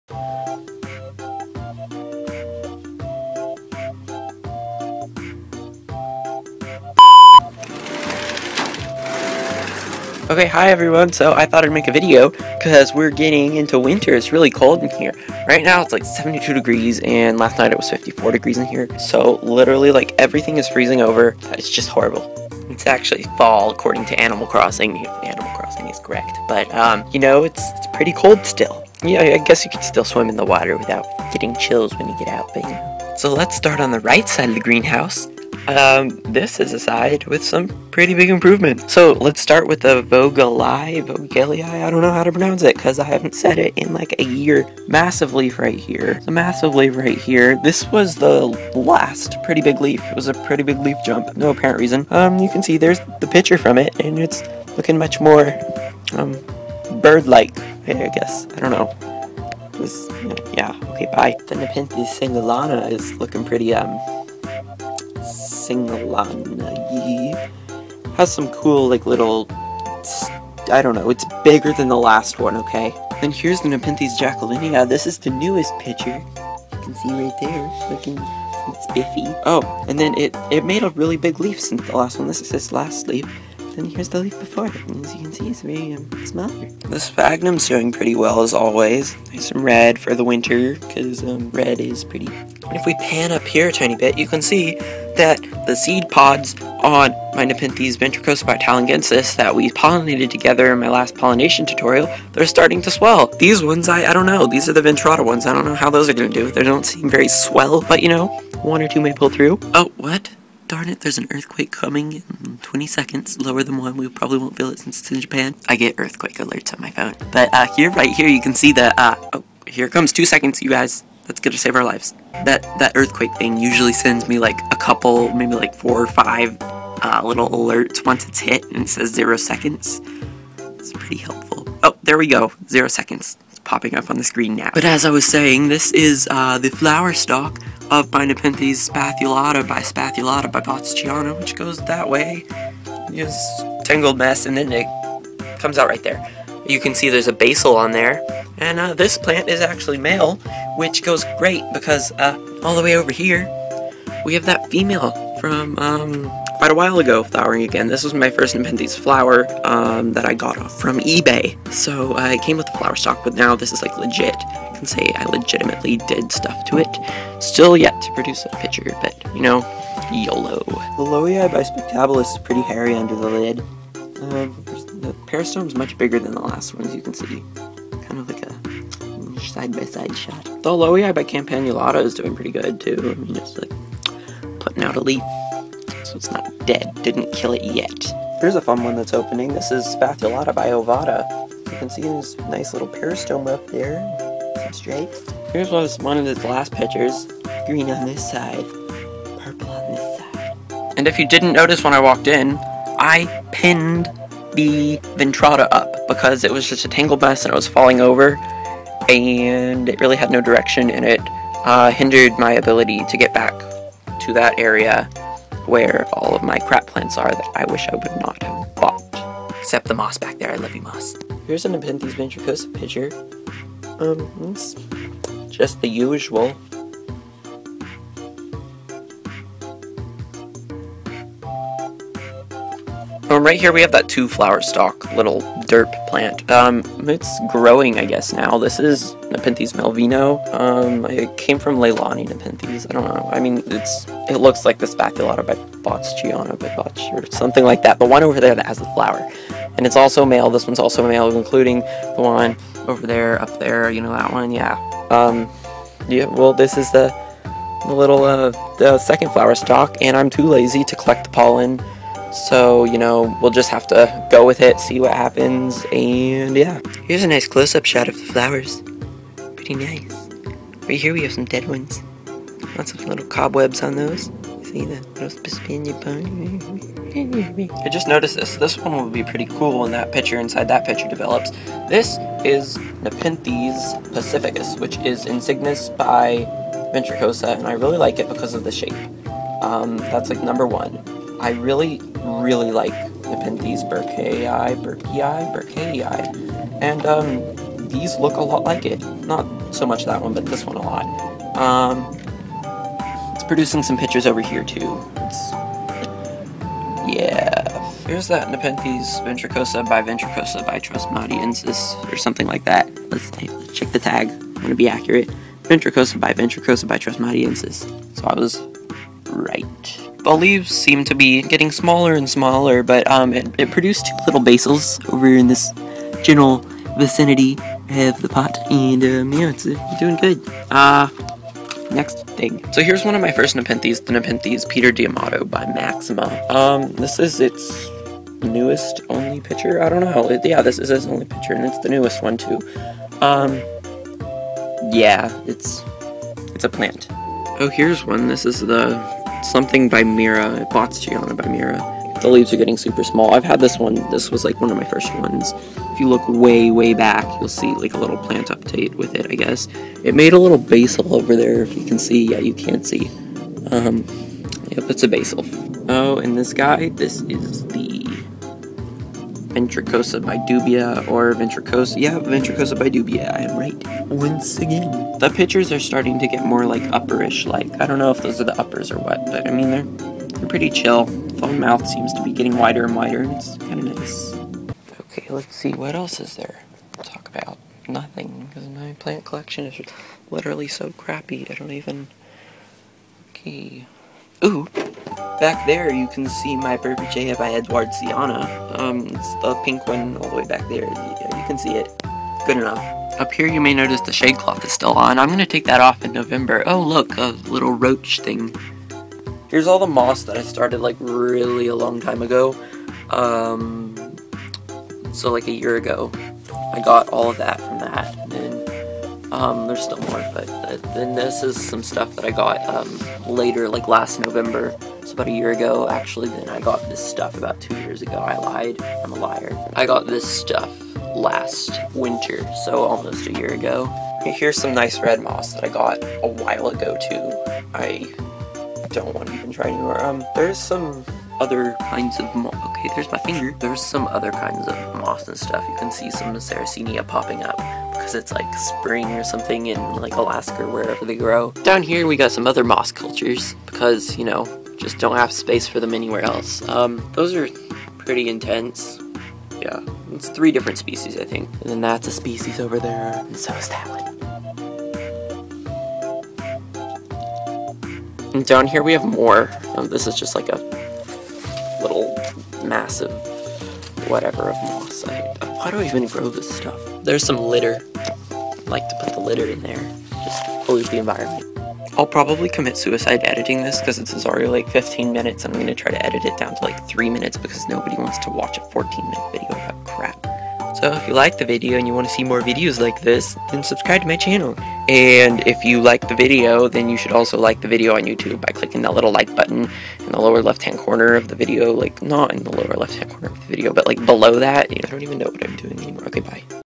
MASSIVE FLESH EATING CARNIVOROUS PLANTS - Nepenthes Update #13
These are Nepenthes tropical pitcher plants from Southeast Asia. I grow lots of them in my tropical greenhouse.

Don't forget to click the LIKE button and FAVORITE the video to let me know you want to see more like this!

My Nepenthes as of 30 October, 2013.